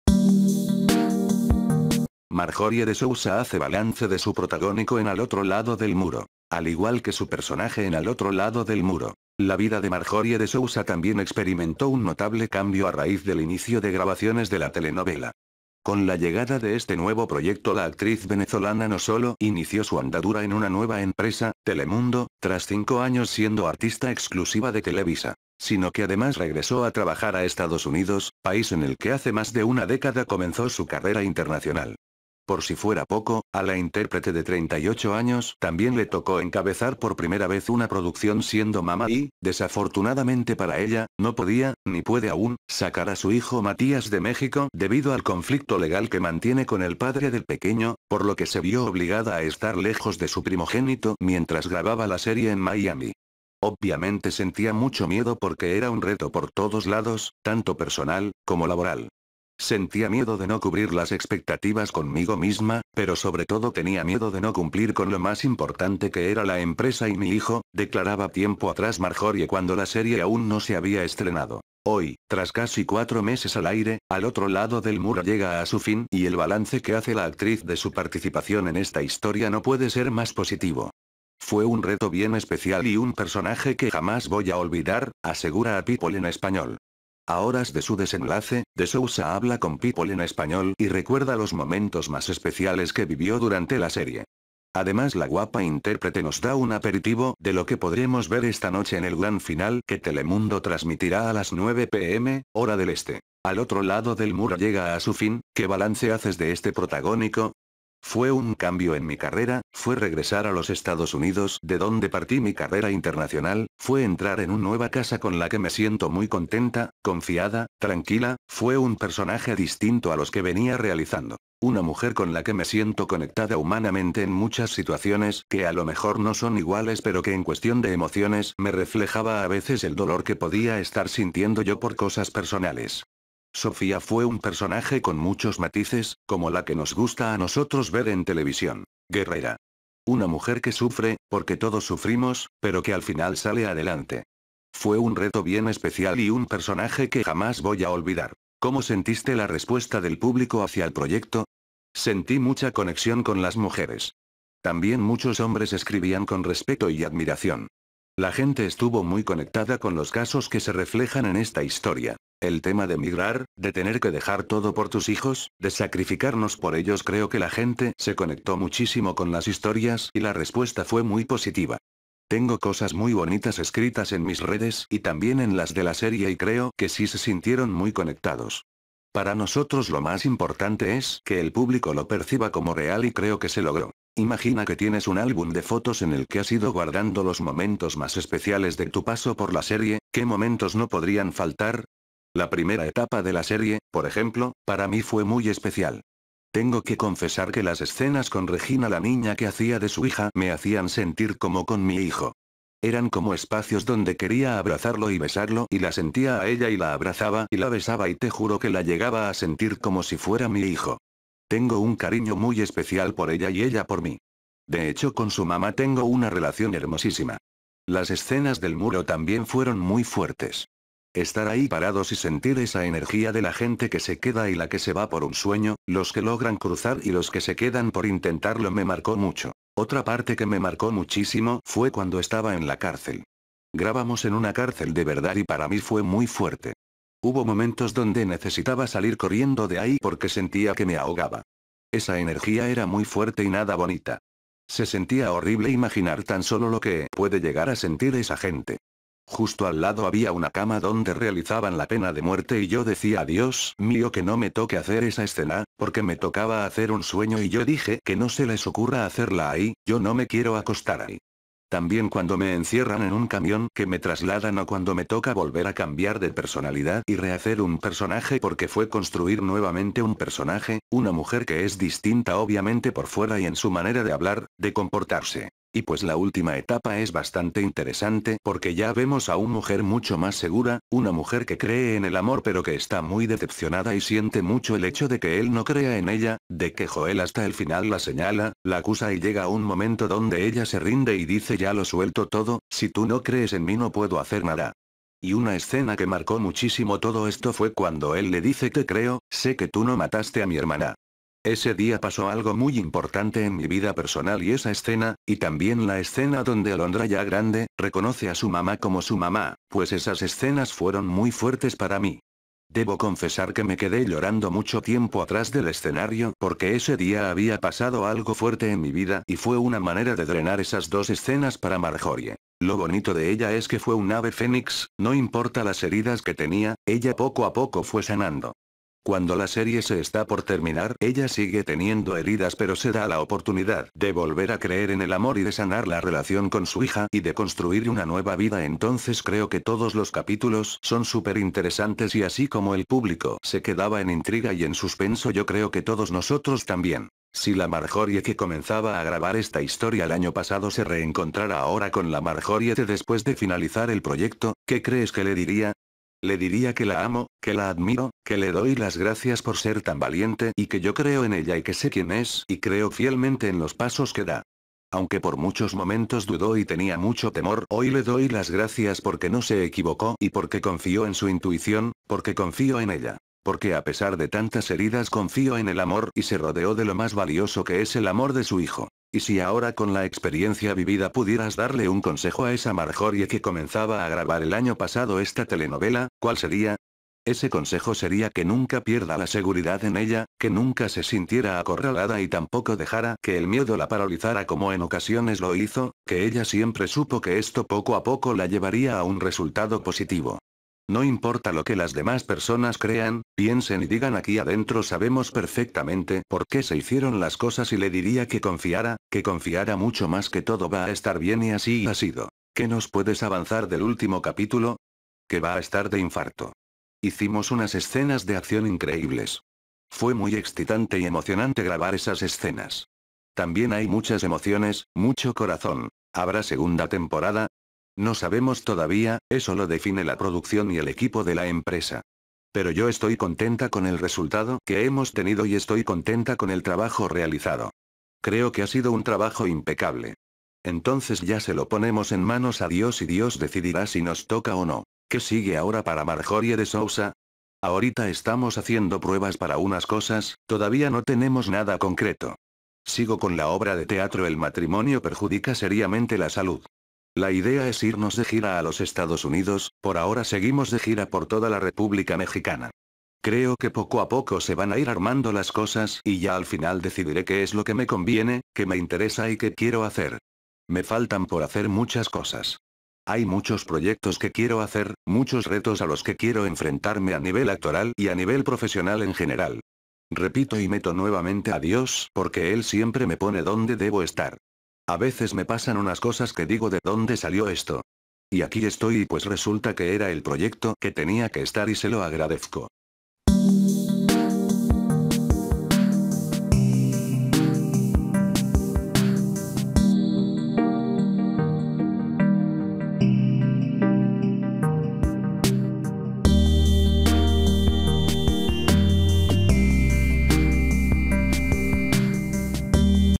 Marjorie de Sousa hace balance de su protagónico en Al otro lado del muro
Al igual que su personaje en Al otro lado del muro; la vida de Marjorie de Sousa también experimentó un notable cambio a raíz del inicio de grabaciones de la telenovela. Con la llegada de este nuevo proyecto la actriz venezolana no solo inició su andadura en una nueva empresa, Telemundo, tras cinco años siendo artista exclusiva de Televisa; sino que además regresó a trabajar a Estados Unidos, país en el que hace más de una década comenzó su carrera internacional. Por si fuera poco, a la intérprete de 38 años también le tocó encabezar por primera vez una producción siendo mamá y, desafortunadamente para ella, no podía, ni puede aún, sacar a su hijo Matías de México debido al conflicto legal que mantiene con el padre del pequeño, por lo que se vio obligada a estar lejos de su primogénito mientras grababa la serie en Miami. “Obviamente sentía mucho miedo porque era un reto por todos lados, tanto personal, como laboral. Sentía miedo de no cubrir las expectativas conmigo misma, pero sobre todo tenía miedo de no cumplir con lo más importante que era la empresa y mi hijo”, declaraba tiempo atrás Marjorie cuando la serie aún no se había estrenado.